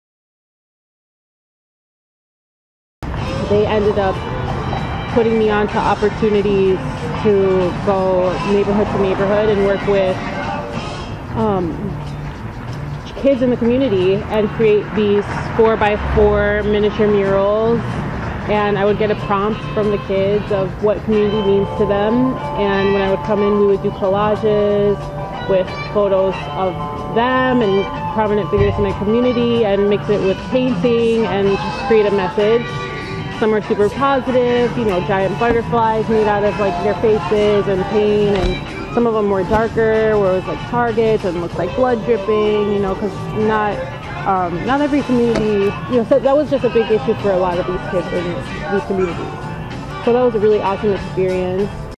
4x4 Chicago Neighborhood Murals
Yasmeen Henderson: Multitalented artist channeling personal vulnerability to re-form community
Meet Yaz. Yasmeen Henderson is an artist and musician living in Milwaukee’s Riverwest neighborhood commissioned through WaterMarks to create an art project for Lindsay Heights. She has survived family trauma and tragedy in a way that positions her to use art to empathize with young people who face struggles that are difficult to express.

“My mom was killed when I was 20,” she says. “So, that was pretty tough. It really just disconnected me from myself, my life.”

She had already left home at age 16—she describes abuse in her childhood household—but before then her mother and grandparents had encouraged her artistic endeavors growing up in Milwaukee. Her passion for singing extended to several choirs throughout her high-school years. Art and music was an outlet.

But her mother’s death and subsequent family home foreclosure shut everything down—creatively and emotionally.

Into her twenties, Yaz eventually found a place in the Milwaukee music community. She started singing in a band and loving it. But the music scene led to a cycle of performance and partying that Yaz says “landslided into a little bit of alcoholism.”

“We were always at some bar getting some drinks for free or whatever,” she says. “I had all this subconscious grief that I had to deal with. I’ve never been a person who has to be outwardly sad. You wouldn’t see me crying at bars. I was partying and focusing on how to have fun and not deal with this ever.”

At one point, a friend moving to New York gave her art supplies including clay, easel, oil paints, and multiple canvases. Armed with these tools, Yaz found herself forming a healthier habit. She let go of the music scene and quit drinking. Making art became her therapy. She would go Live on Facebook and just draw. Drawing led to painting. Painting led to more complex pieces.

“The longer my sobriety continued, the more intricate my art was getting and I started painting more. That became a very positive outlook for me, something to focus on.”

Part of her grief journey included creating pieces that could not be contained within a conventional square canvas. In one instance Yaz wanted to cut out the shape of a shark she was making for her husband. “I went to Bliffert’s and I was like, hey, you guys have a saw I can use?” Her inquiry led to someone who let her use his saw and was so impressed with her newfound prowess that he gifted the saw to her. The saw became an instrument of power, craft, and creativity.

Yaz kept creating. She noticed, though, that what she was painting was characters that were vividly colored but who were suppressing emotion. “They were just not giving anything, all super devoid of emotion,” she says. “It was a reflection of my need to not feel all [that] negative pain, that mask I wore my entire life, at school, with friends, at home.”

During the pandemic, Yaz moved from Milwaukee to Chicago where she scoured the scene for artists and allies. She gained an introduction to Sam Kirk, prominent Chicago-based muralist, whose large-scale vibrant murals represent LGBTQIA communities. The connection led Yaz to a project working with Chicago neighborhood kids to create 4-by-4-foot mini-murals that reflected what they thought and felt about their communities. She worked with kids from age 3 to 17 in a number of neighborhoods over several months.

In Chicago, she found a passion for working with kids, many of whom were facing trauma that feels inescapable—lost loved ones, drug addictions, gun violence, the grind of racism and poverty. While their mini-murals were objects of communal uplift, Yaz says they also allowed the kids a safe space for expressing previously unexpressed fears.

“It is a big part of the puzzle figuring out how you stay joyful when you are in a negative situation growing up,” Yaz says. “I went through all of the trial and error of that, and I think being able to work with kids in that way—subtly about those things—art is a great outlet for that.”

Yaz loved her time in Chicago and felt supported as an artist. People were buying her pieces. She felt involved making a difference and appreciated by the artistic community. But Chicago is also expensive, and ultimately her road led back to Milwaukee, a city with which she has a complicated “bittersweet” relationship—bitter in the past and now working on the sweet.